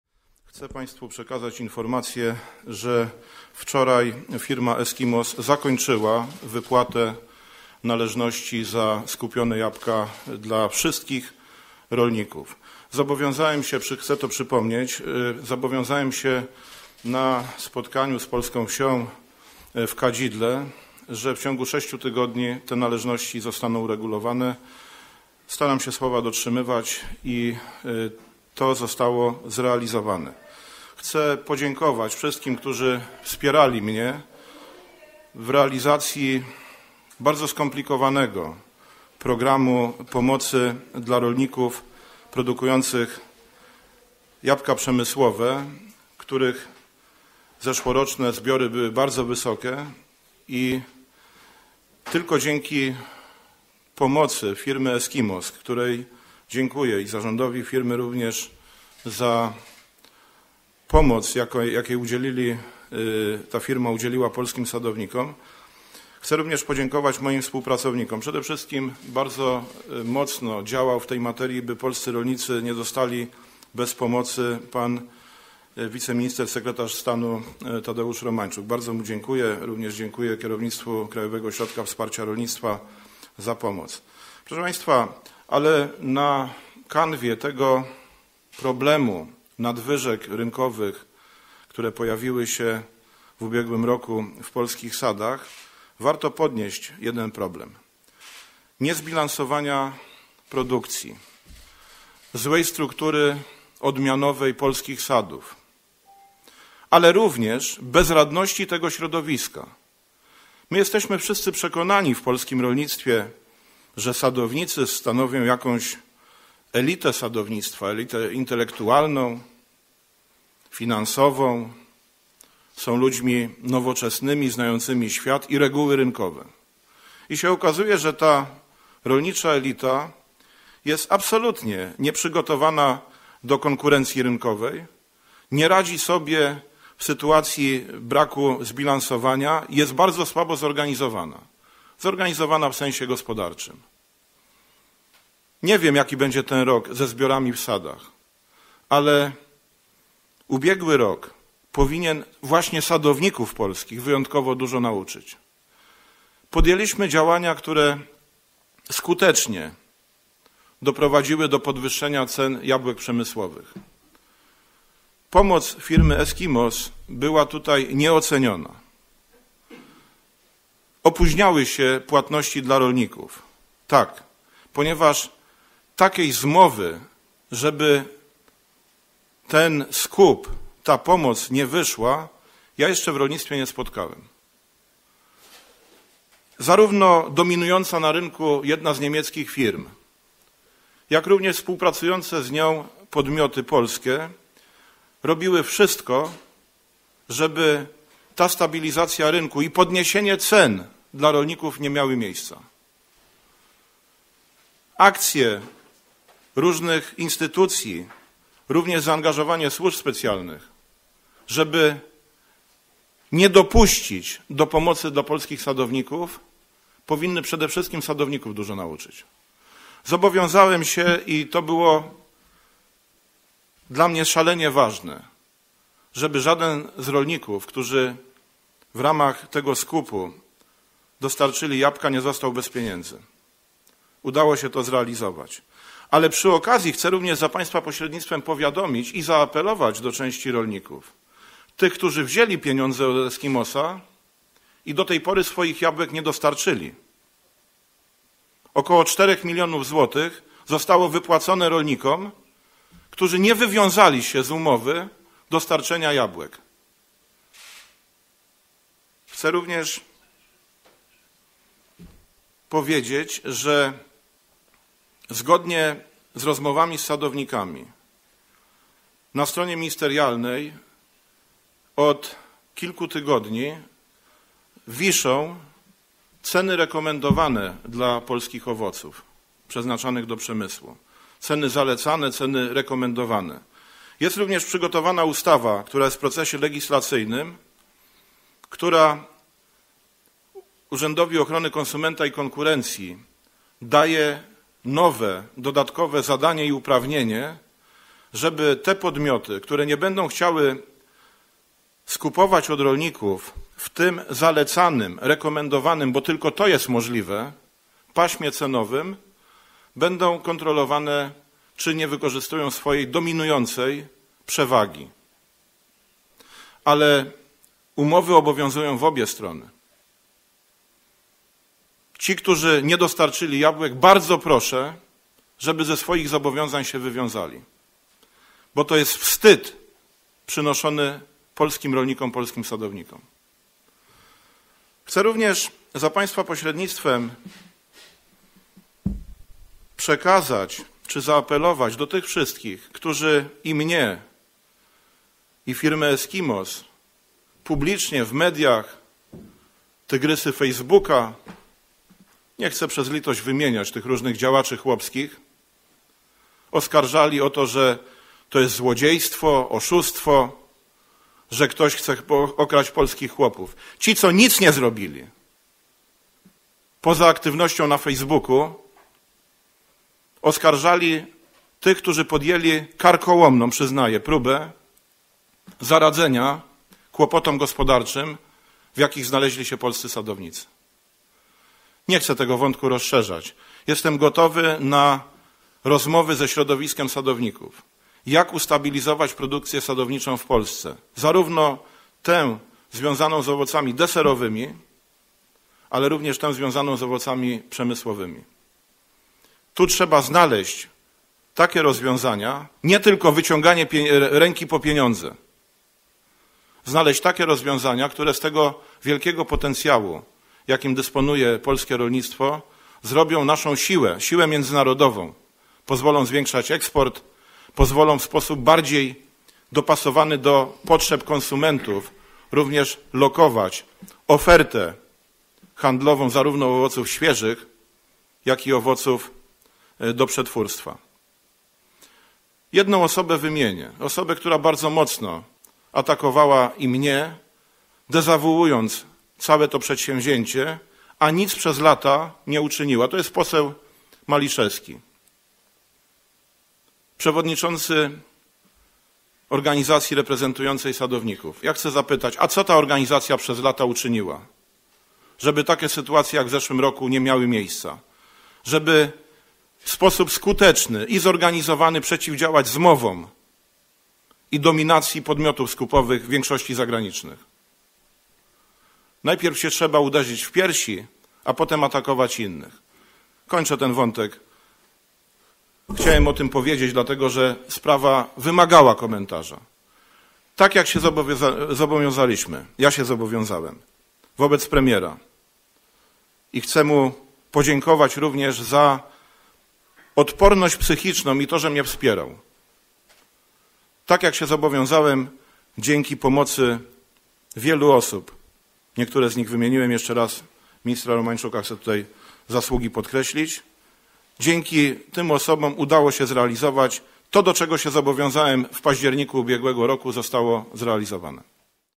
Pieniądze za jabłka wypłacone - akcja stabilizacji cen na rynku jabłka przemysłowego udana
Minister rolnictwa i rozwoju wsi Jan Krzysztof Ardanowski poinformował dziś, że firma Eskimos, która podjęła się skupu jabłek uregulowała wszystkie swoje zobowiązania wobec sadowników.

– Ta bardzo skomplikowana operacja rynkowa, związana z tragiczną sytuacją w skupie jabłek, została zakończona powodzeniem. Zgodnie ze składanymi obietnicami wszyscy, którzy dostarczyli jabłka otrzymali za nie zapłatę – zapewnił minister.

– Takiej zmowy, aby ten skup nie wyszedł jeszcze nie widziałem – podkreślił minister i dodał, ze to powinno przede wszystkim czegoś nauczyć samych sadowników.

Szef resortu zwrócił przy tym uwagę, że są jednak i tacy producenci, którzy nie wywiązali się z umowy i nie dostarczyli jeszcze jabłek.
Informując o zakończeniu wypłat minister podniósł kwestię przyczyn problemów na rynku jabłek.

– Mamy do czynienia z brakiem zbilansowania produkcji, brakiem przygotowania do konkurencji rynkowej i ze słabym zorganizowaniem gospodarczym samej branży – powiedział szef resortu rolnictwa pytając jednocześnie co w tej kwestii uczynili sami sadownicy, którzy do tej pory uważani byli za rolniczą elitę.

Podsumowując całą akcję prowadzoną przez firmę Eskimos minister zwrócił uwagę, że spowodowała ona podniesienie cen skupu jabłek do poziomu akceptowalnego przez środowisko.